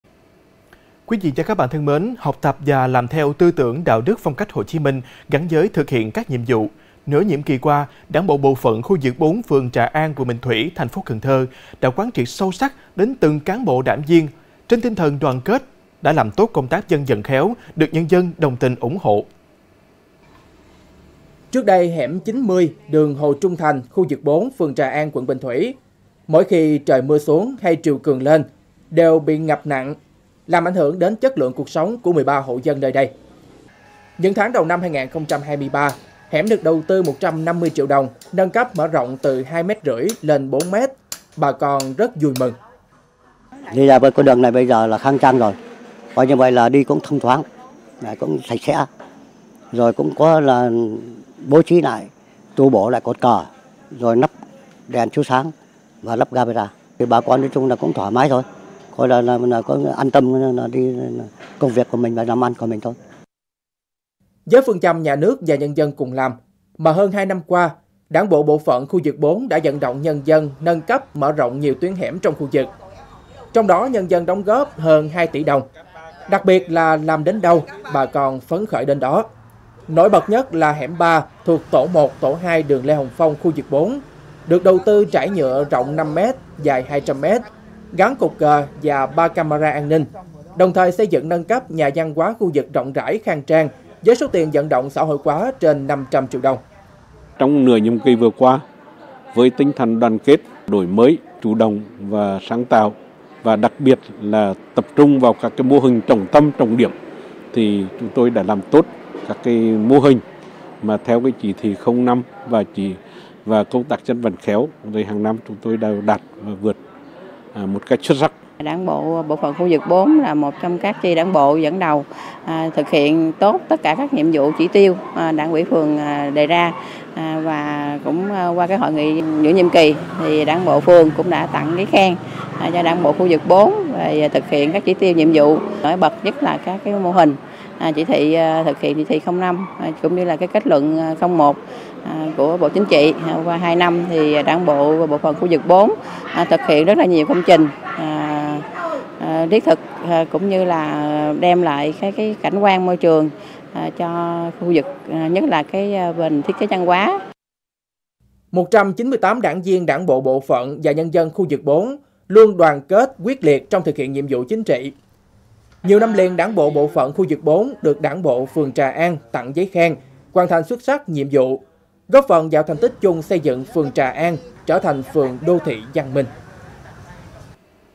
Phát huy sức dân chỉnh trang đô thị | Cần Thơ TV
Học tập và làm theo tư tưởng, đạo đức, phong cách Hồ Chí Minh gắn với thực hiện các nhiệm vụ, nửa nhiệm kỳ qua, Đảng bộ bộ phận khu vực 4, phường Trà An, quận Bình Thủy, TP. Cần Thơ đã quán triệt sâu sắc đến từng cán bộ, đảng viên trên tinh thần đoàn kết, đã làm tốt công tác dân vận khéo được nhân dân đồng tình ủng hộ.